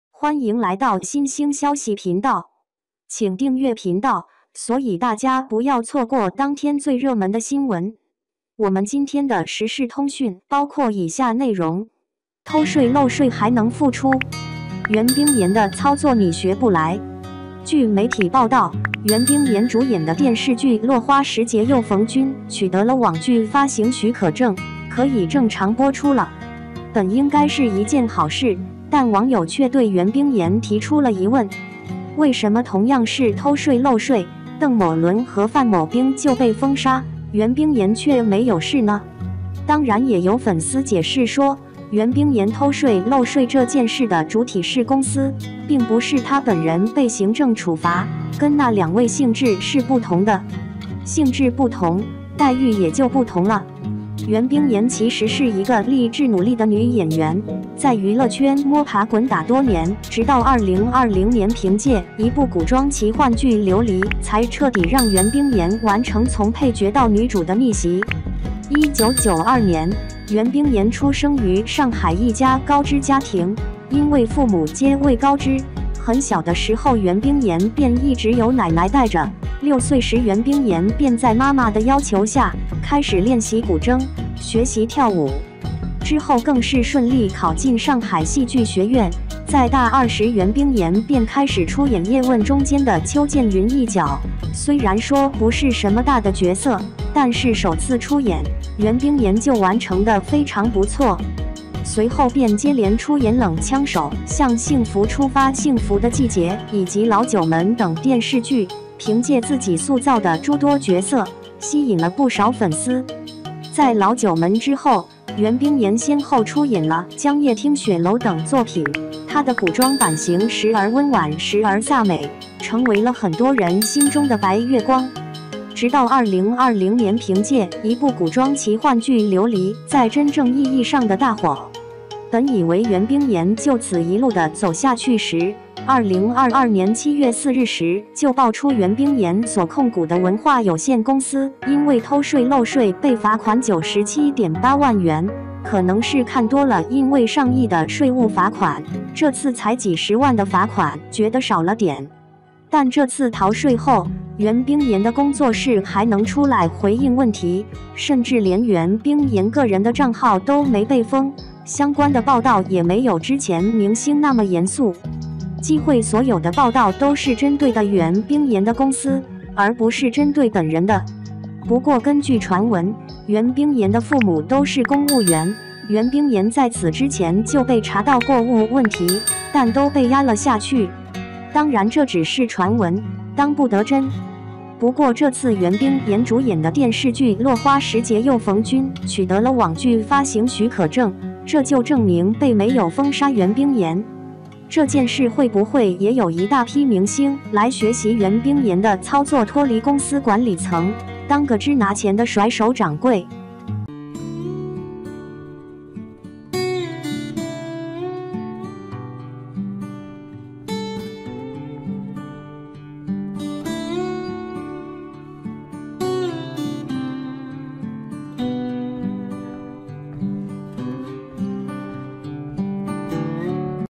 偷税漏税还能复出？袁冰妍的操作你学不来
偷税漏税还能复出？袁冰妍的操作你学不来 | 新星消息